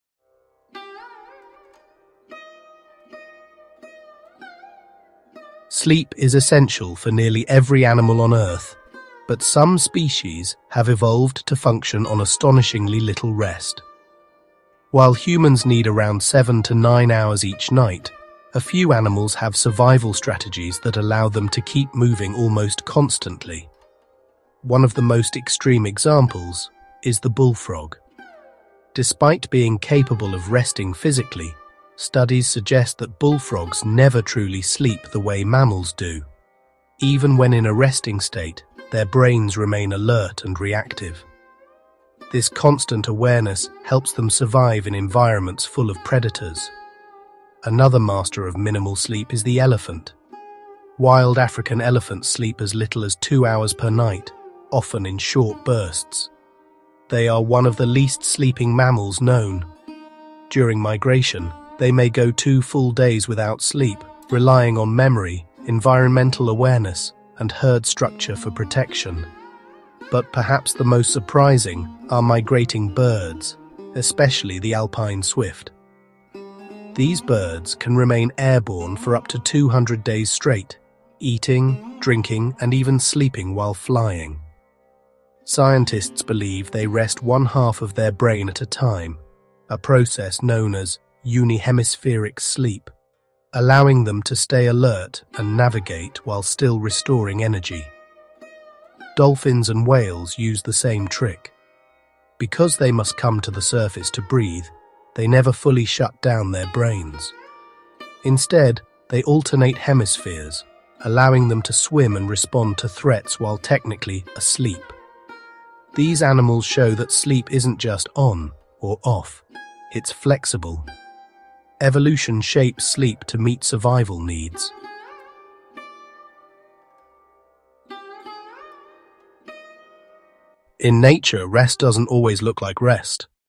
Animals that don’t need to sleep
These Animals Hardly Sleep—Here’s How They Survive: Explore surprising adaptations in animals that sleep the least—from bullfrogs that stay neurologically alert to elephants surviving on just a couple hours, and Alpine swifts that sleep while flying. Learn about unihemispheric sleep in birds, dolphins, and whales, and how evolution shapes rest for migration, predator avoidance, and group safety. Perfect for curious science learners who love wildlife facts, sleep biology, and animal behavior.